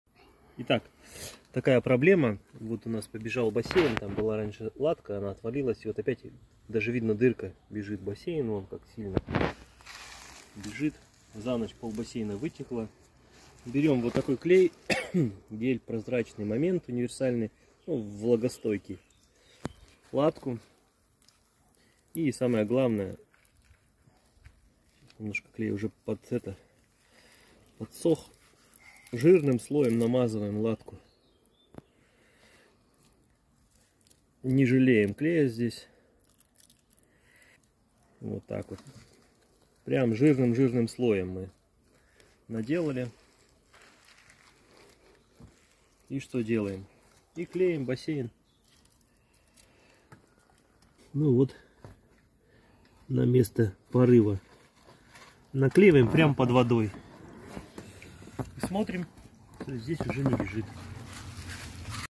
Ремонт каркасного бассейна не сливая воду.Repair of a frame pool without draining the water.
иногда можно испытать стресс когда налил бассейн и увидел что он бежит. Ведь чтобы заклеить нужно слить воду, НЕТ в этом видео показано как заклеить бассейн не сливая воды для ремонта использовали клей момент влагостойкий.
sometimes you can feel stress when you fill the pool and see him running. After all, in order to glue you need to drain the water, NO in this video it is shown how to glue the pool without draining the water for the repair we used the moisture-resistant glue.